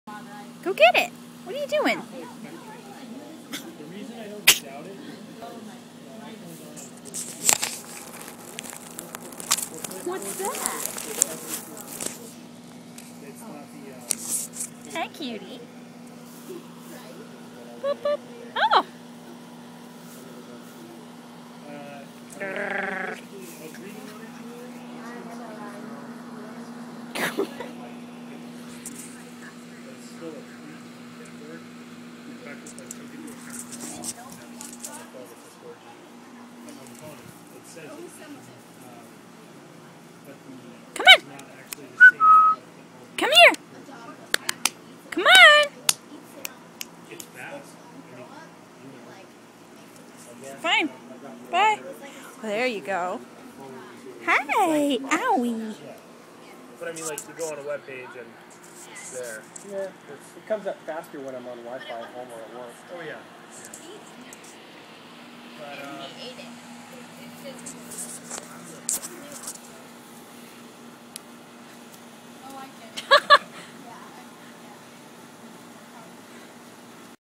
Baby Cardigan Welsh Corgi - 11 weeks old
Scout in the backyard. This was taken when she was about 10-12 weeks old.